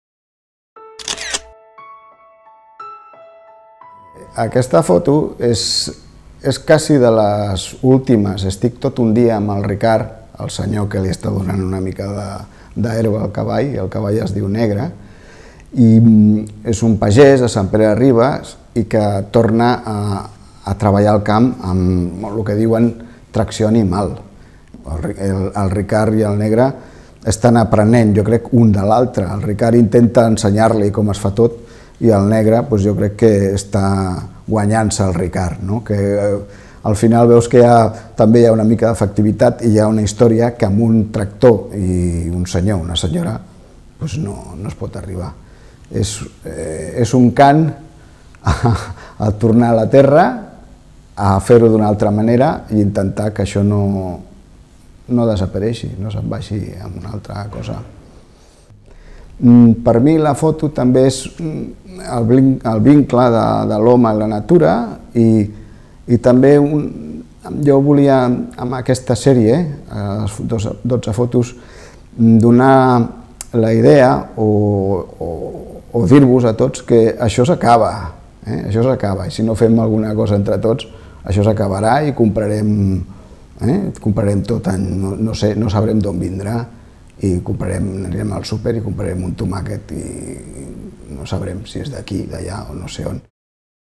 TORNAR ALS ORÍGENS | Marcel·lí Sàenz
TORNAR ALS ORÍGENS. 14/02 Sant Pere de Ribes (Garraf) | 17:48 h

Ricard Huguet treballa l’horta de la seva finca de Les Casetes d’en Portacreu, recuperant la tracció animal i ajovant el Negre, un cavall pirinenc català. L’interès creix per motius econòmics i mediambientals.

VOLVER A LOS ORÍGENES. Ricard Huguet trabaja el huerto de su finca de Les Casetes d’en Portacreu, recuperando la tracción animal y enseñando a Negre, un caballo pirenaico catalán. El interés crece por motivos económicos y medioambientales.

BACK TO THE ROOTS. Ricard Huguet is tending to the vegetable garden of his estate in Les Casetes d'en Portacreu, recovering animal traction and training Negre, a Pyrenean Catalan horse. Interest is growing for economic and environmental reasons.

2014 © 1D1F | VínculoVisual